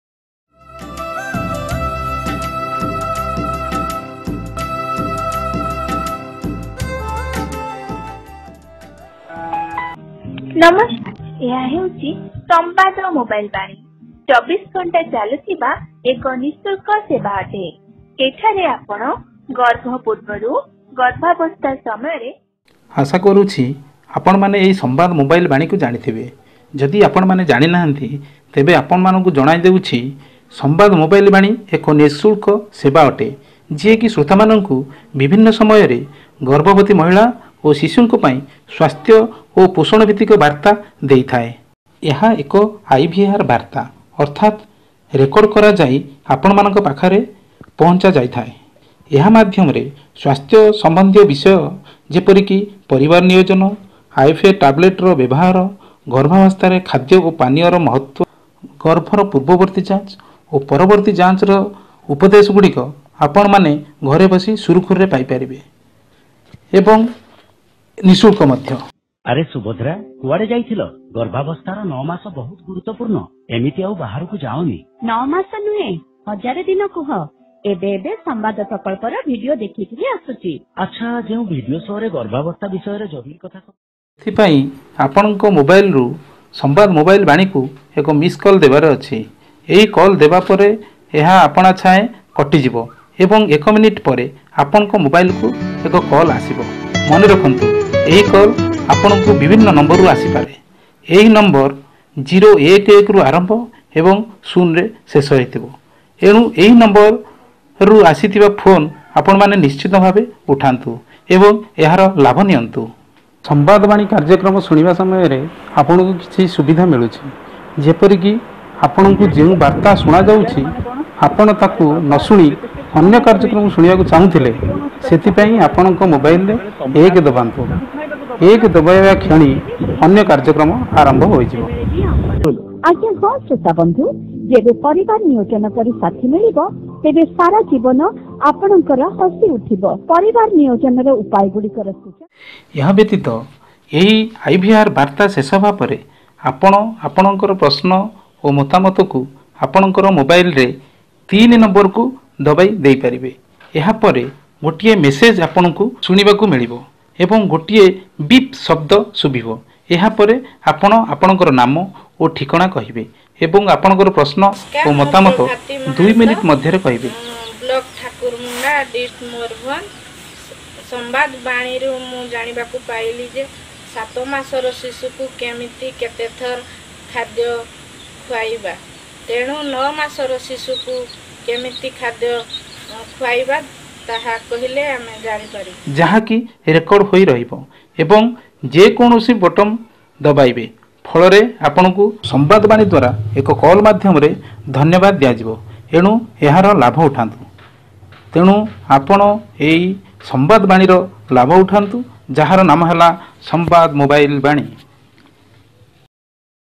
How to use SAMVAD mobile Vani_Odia
This video showcase how to use Samvad Mobile Vani(SMV) the IVR service of SAMVAD project which aims to educate, empower and sensitize the usage of the service.